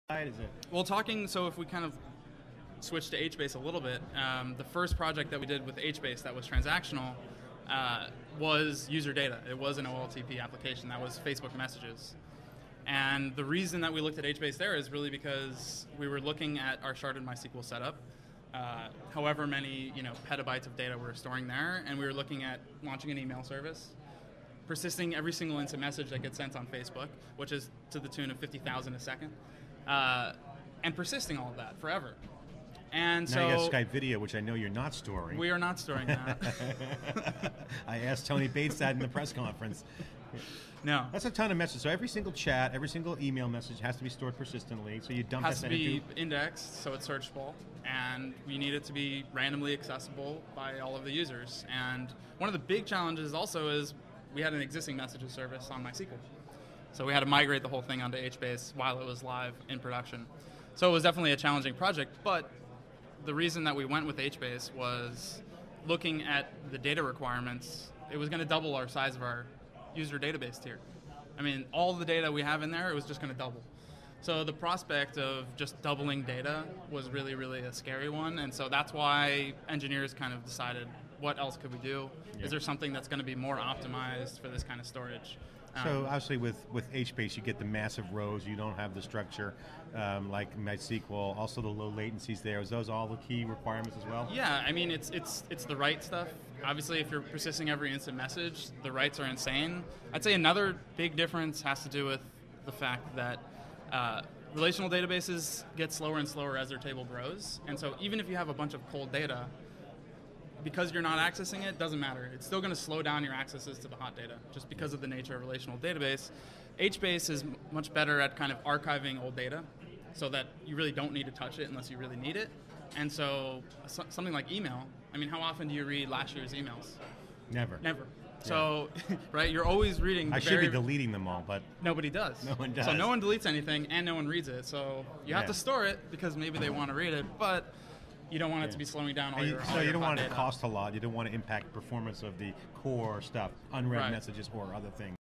Facebook's Gray on HBase Migration, Use Case
Facebook's Jonathan Gray said Facebook decided to migrate from MySQL to HBase to store email and instant messages because of performance and scale concerns. Speaking live inside theCUBE at Hadoop World 2011, Gray said HBase is better at managing hot and cold data.